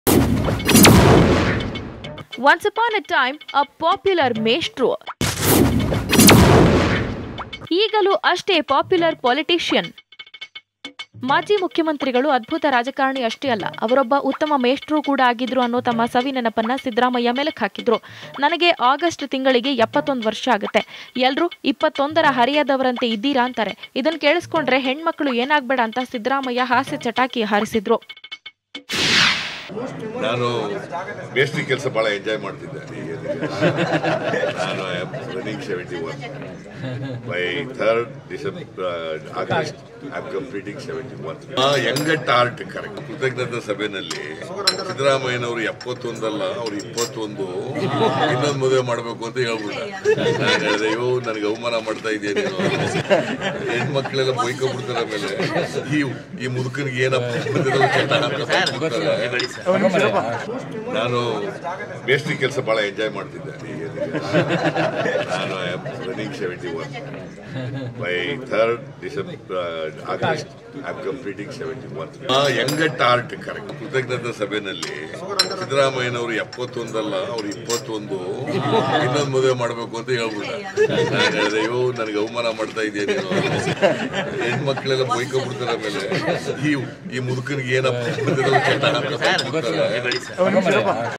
ಹಾಸ್ಯ ಚಟಾಕಿ ಹಾರಿಸಿದ ಸಿದ್ದು,"21ರ ಹರೆಯದವರಂತೆ ಇದ್ದೀರಾ ಅಂತಾರೆ,ಹೆಣ್ಣು ಮಕ್ಕಳು ಕೇಳಿಸಿಕೊಂಡ್ರೆ ಏನಾಗಬೇಡ"
ಇನ್ನು 21ರ ಹರೆಯದವರಂತೆ ಇದ್ದೀರಾ ಅಂತಾರೆ. ಹೆಣ್ಣು ಮಕ್ಕಳು ಕೇಳಿಸಿಕೊಂಡ್ರೆ ಏನಾಗಬೇಡ, ಹಾಸ್ಯ ಚಟಾಕಿ ಹಾರಿಸಿದ ಸಿದ್ದು